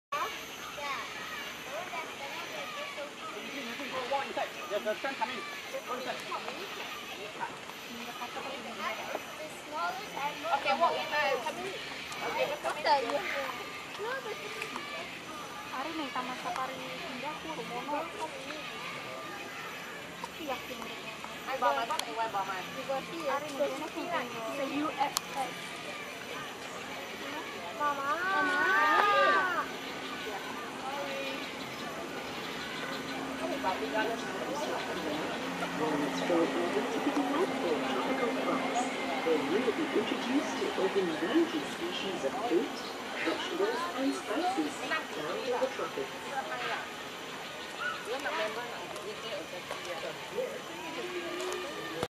Meerkats at the Singapore Zoo (720p HD)
The exhibit home to the meerkat, which is the smallest of the mongoose family, living Kalahari Desert in Botswana, in much of the Namib Desert in Namibia and southwestern Angola, and in South Africa. Not to be confused with Timon, the cartoon meerkat from Disney's "The Lion King". Part of Wild Africa.